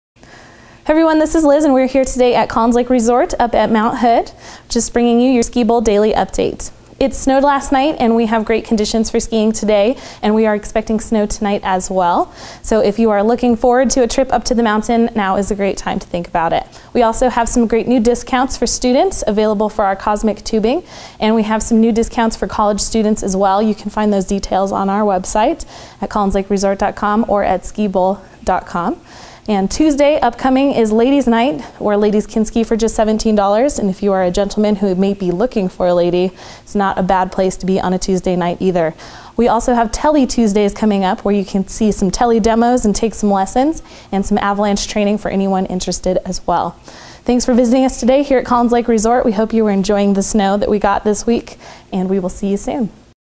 Skibowl Daily Conditions Update February 20th 2012
Today's Daily Update comes from Liz over at Collins Lake Resort! We've got new discounts, new specials, and if you're looking for a place to spend the night check out Collins Lake Resort!